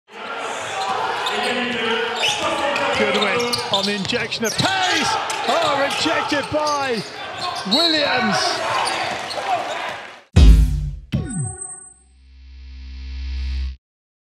7DAYS EuroCup Block of the Round: JaCorey Williams, Dolomiti Energia Trento
JaCorey Williams stops Archie Goodwin in a critical moment of the game - what a block!